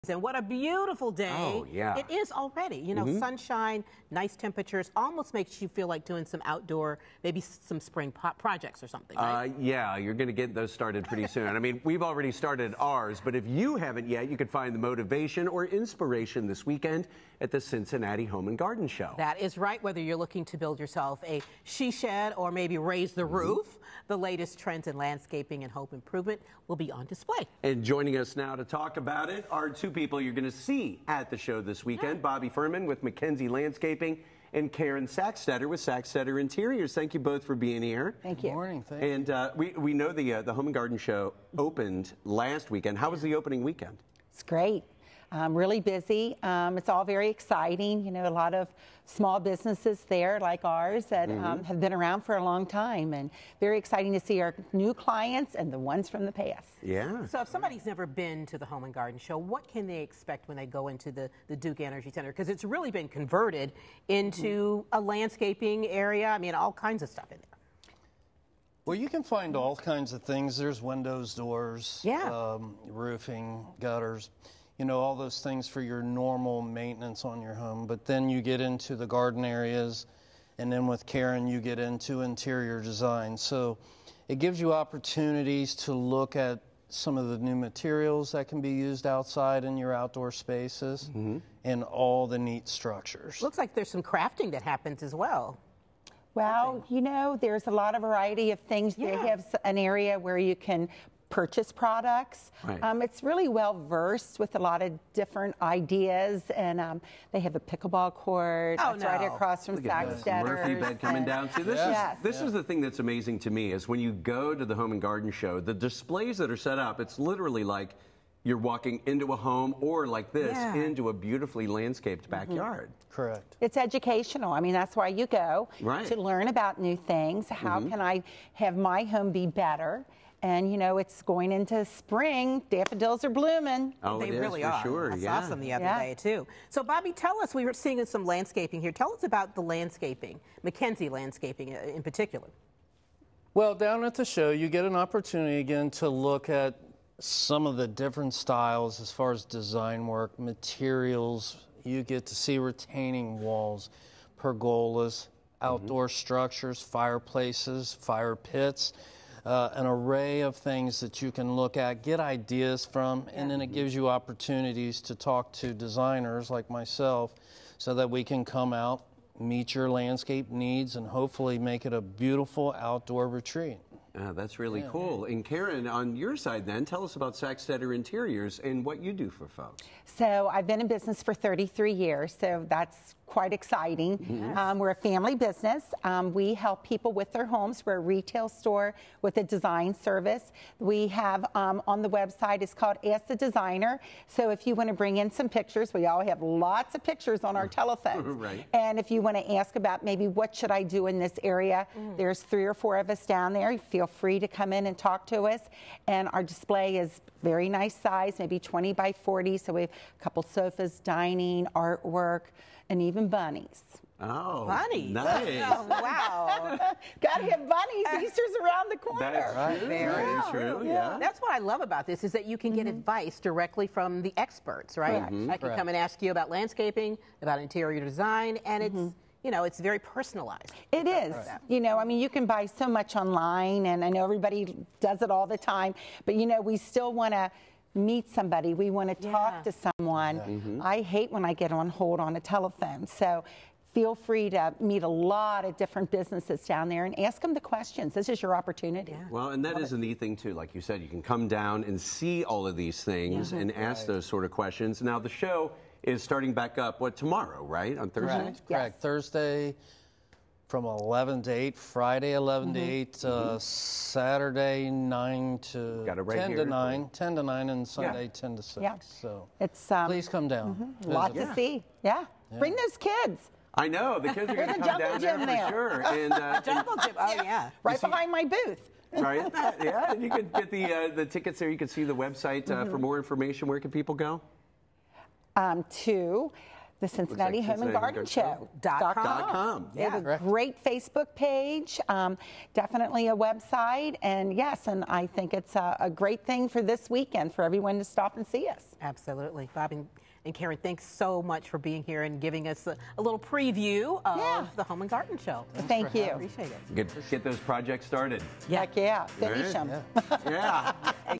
Cincinnati Home + Garden Show | Cincy Lifestyle
The Cincinnati Home and Garden Show is back for its 54th year! Find the latest trends in landscaping and home improvement at the Duke Energy Convention Center.

Bobby Fuhrman from Mckenzie Landscaping and Karen Sacksteder from Sacksteder Interiors joined Michelle Hopkins and Pete Scalia to tell us more!

The Cincinnati Home + Garden Show is open the following hours:
Thursday, March 2, 11am-8pm
Friday, March 3, 11am-8pm
Saturday, March 4, 10am-9pm
Sunday, March 5, 10am-6pm